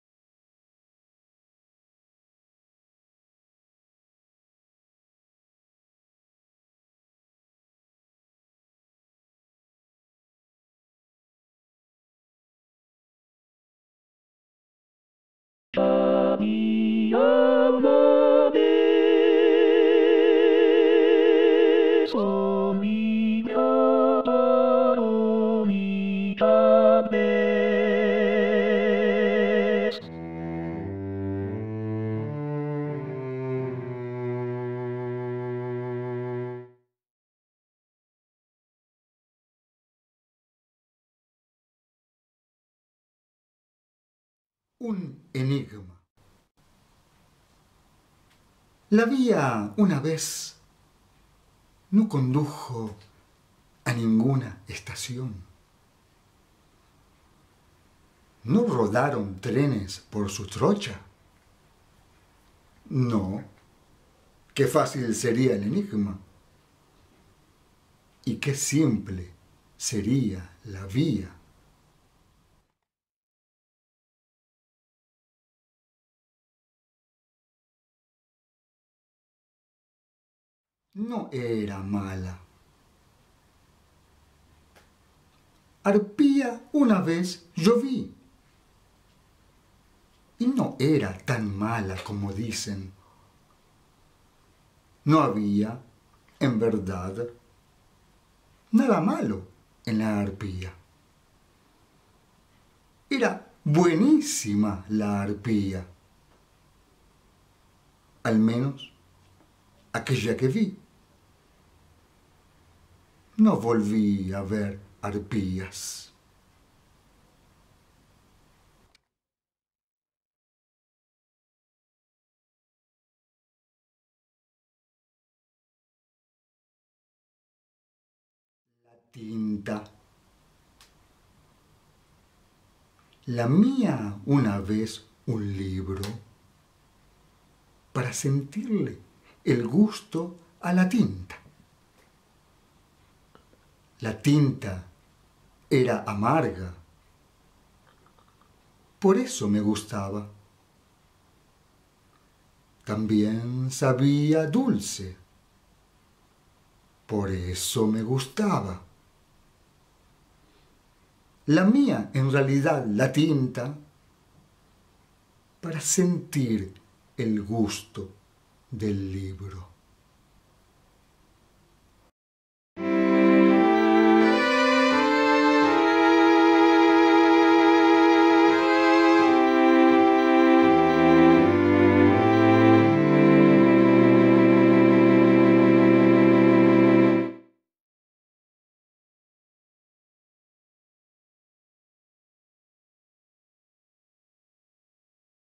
David Wapner lee "Cabía una vez"
Tres poemas del libro "Cabía Una Vez", de David Wapner, con ilustraciones de Juan Lima, editado por editorial Calibroscopio, Buenos Aires, diciembre de 2013.